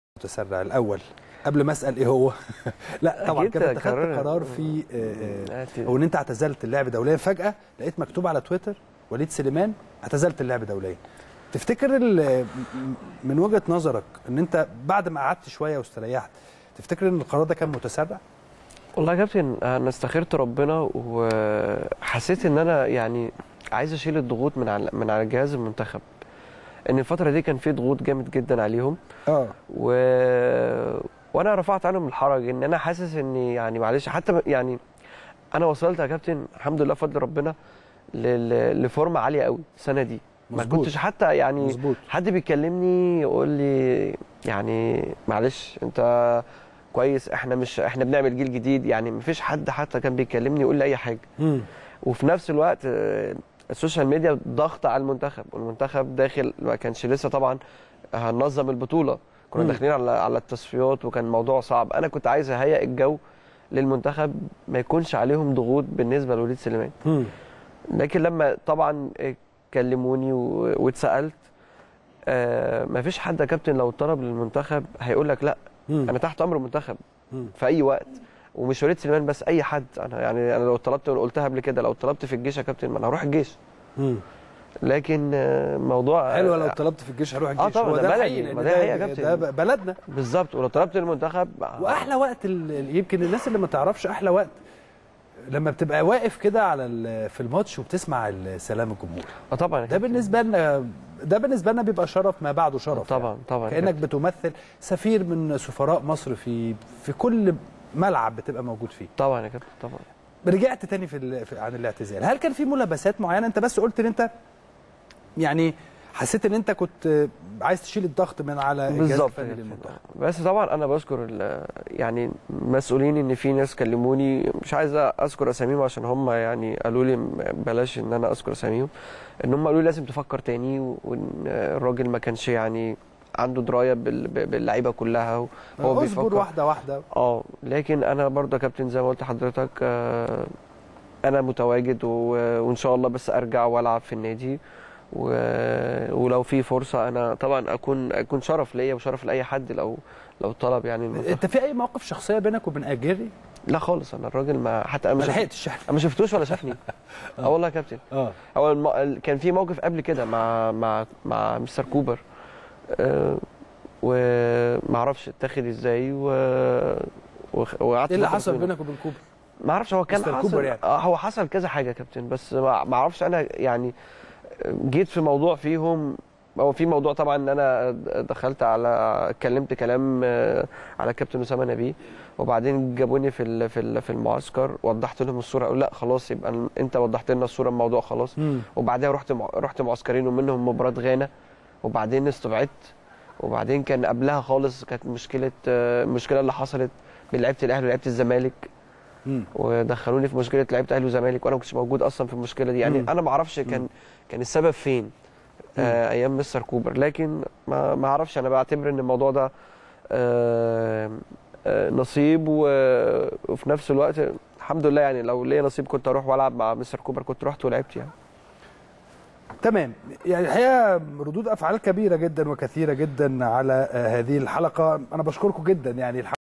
وليد سليمان: خدمة منتخب مصر مثل خدمة الجيش وهذه هي حقيقة خلافى مع كوبر واسامة نبيه
التردد: 12341 أفقي.
القمر: نايل سات.
معامل الخطأ: 5/6.
معدل الترميز: 27500